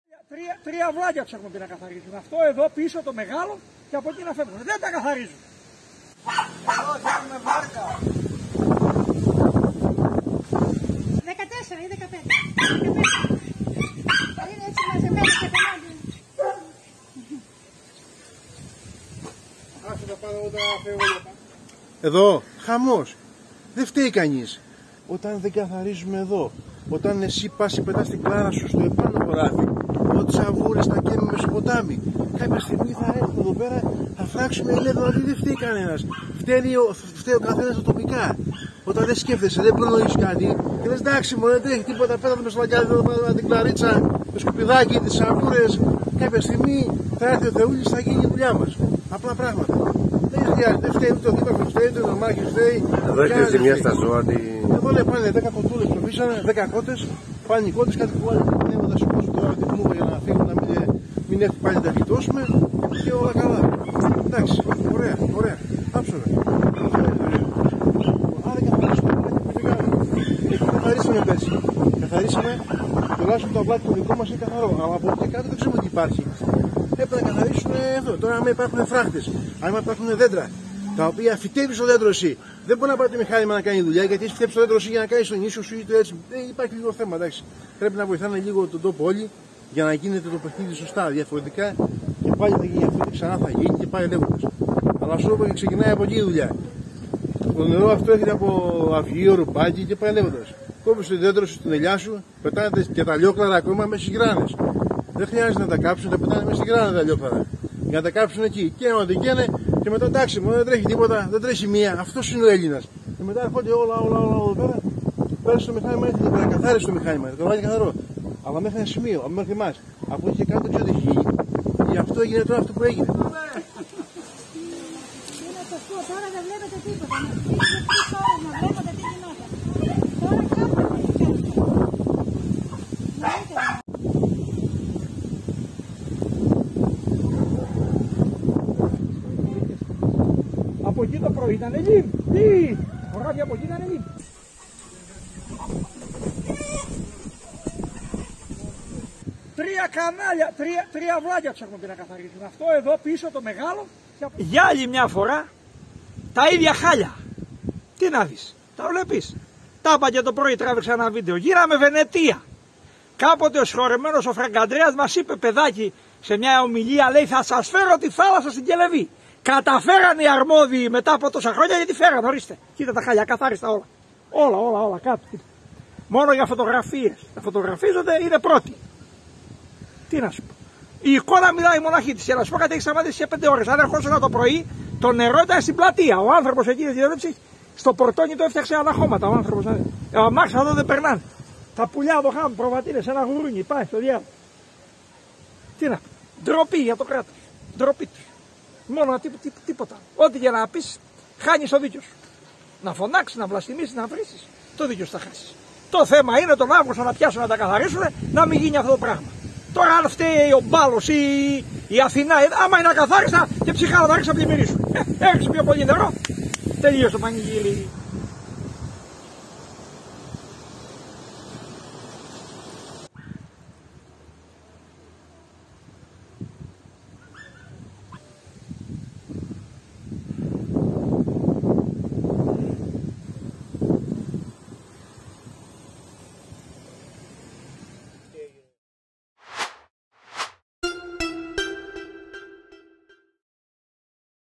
ilialive.gr - Μια απέραντη λίμνη ο Κόροιβος Δήμου Πηνειού
ilialive.gr - Μια απέραντη λίμνη ο Κόροιβος Δήμου Πηνειού - 15 Οκτ 2021